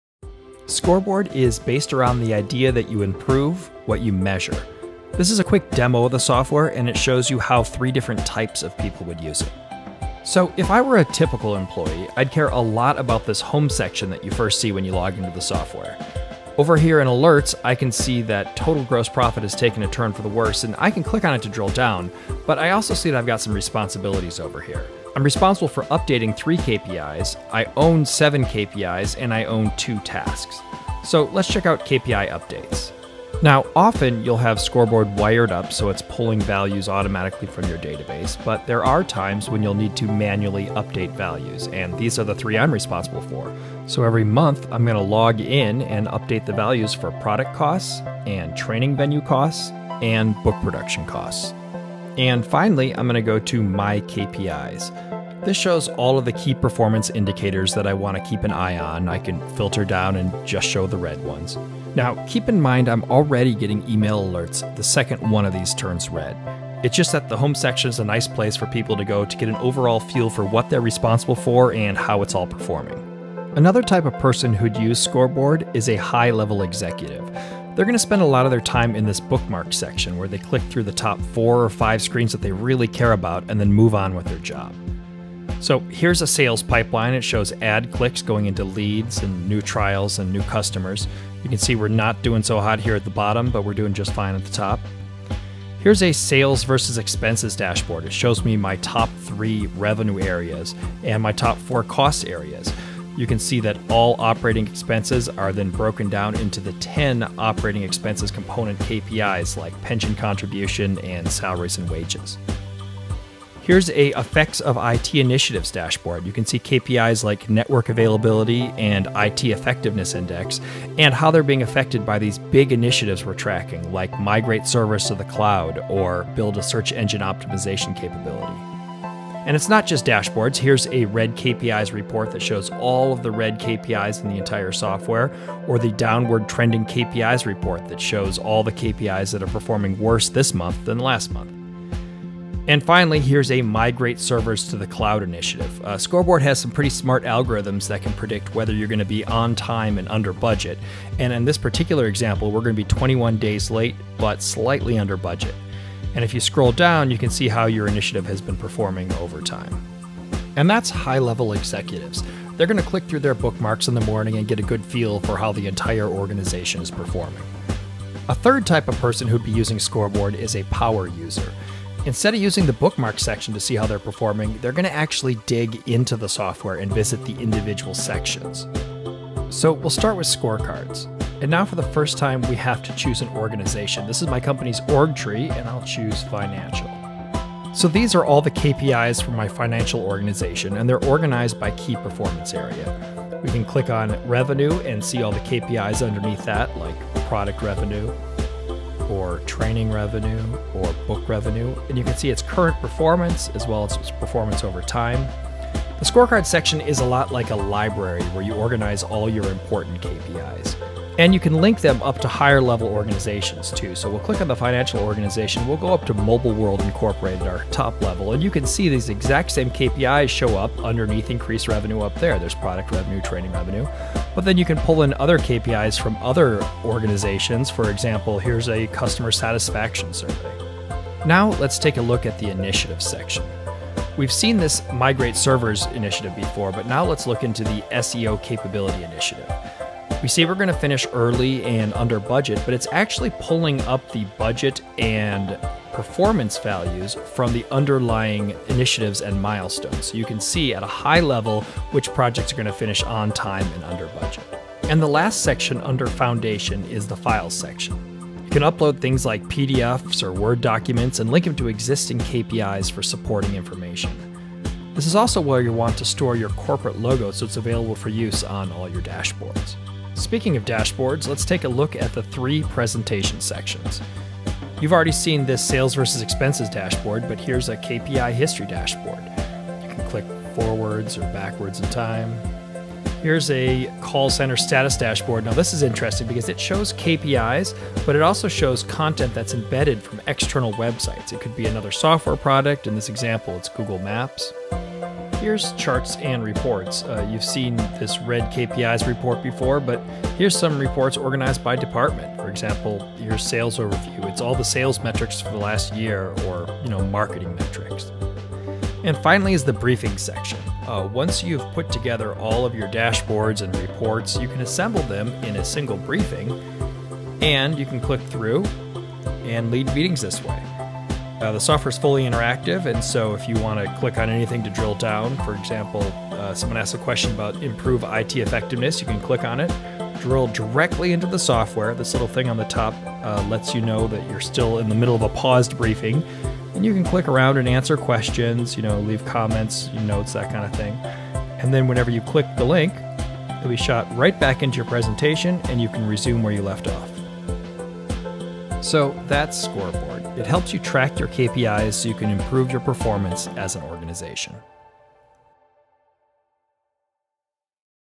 Scoreboard Overview
This is a 6 minute tour of Scoreboard. It covers every section in Scoreboard and shows how three different types of people use the same KPI dashboard software.